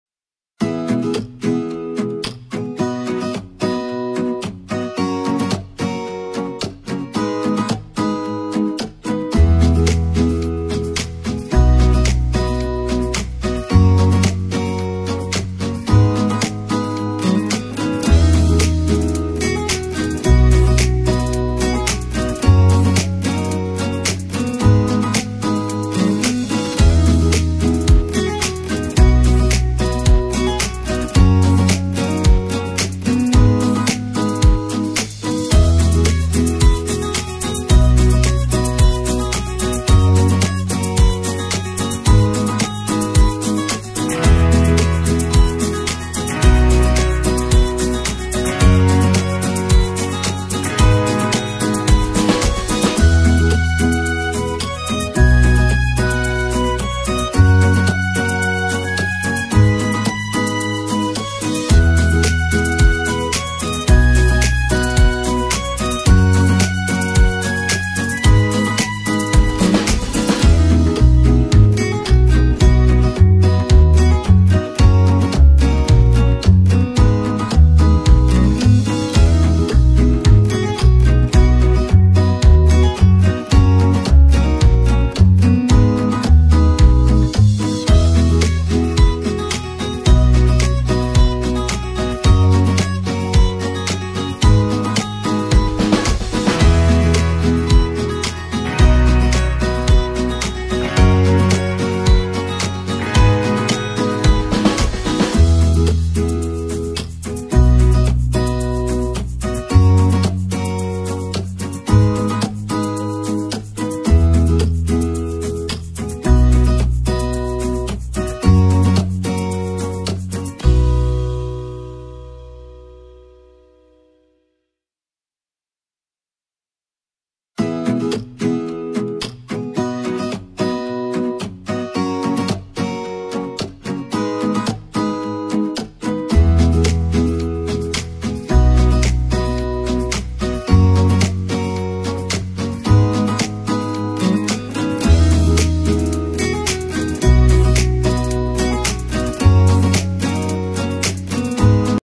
Phuket, Thailand 2023
Phuekt,Thailand. some pics of an Amazing trip for the first time going here @TravelWithUsGJ-bu6cs